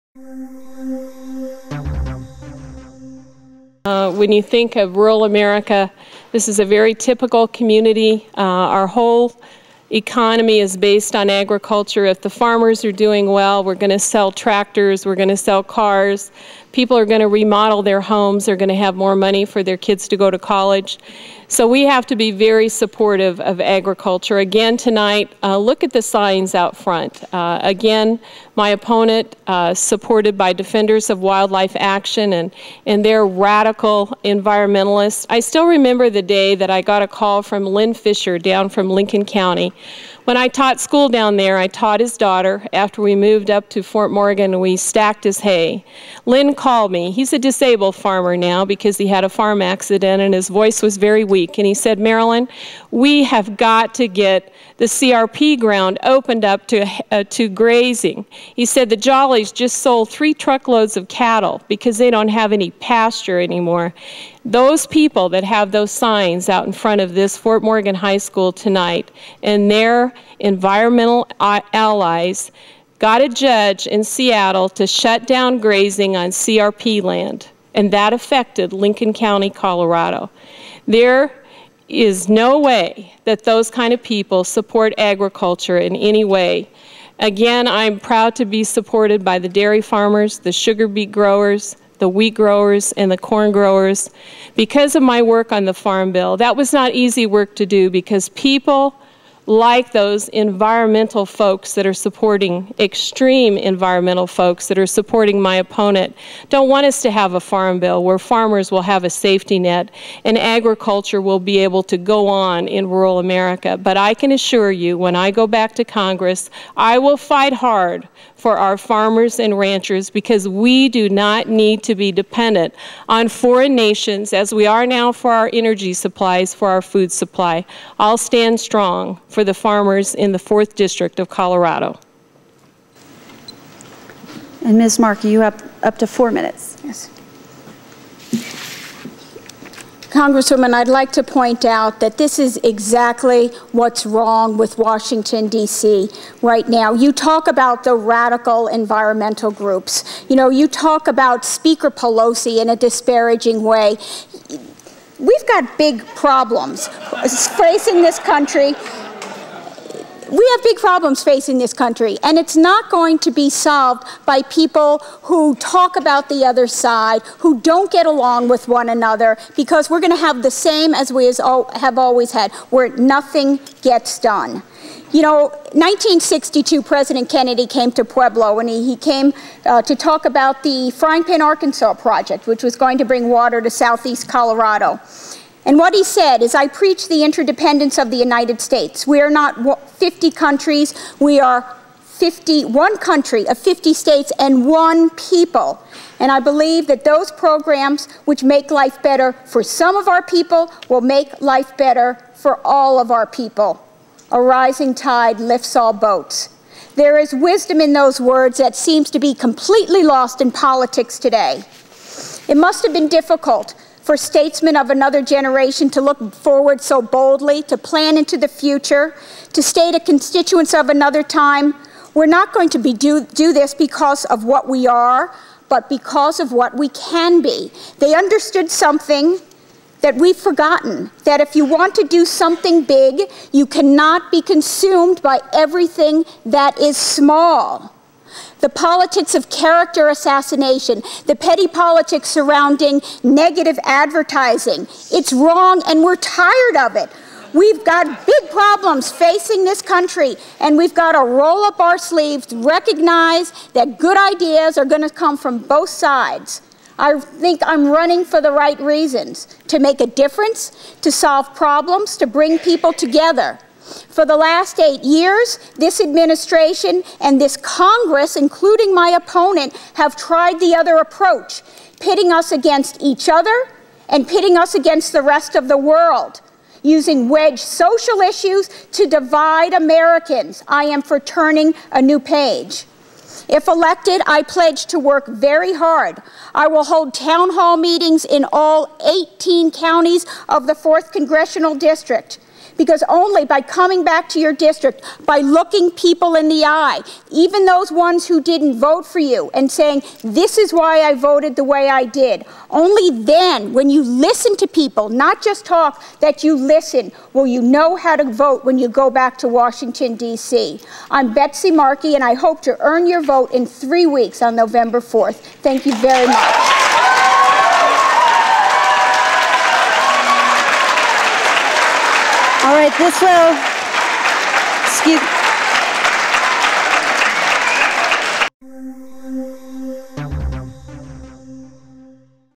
Musgrave v. Markey, CO CD4, Closing Statements
On Monday, 13 March, in the small town of Ft. Morgan, Congresswoman Marilyn Musgrave and challenger Betsy Markey debated at Ft. Morgan High School.  The beginning of Ms. Musgrave's statement is cut off slightly due to technical complications.  My apologies.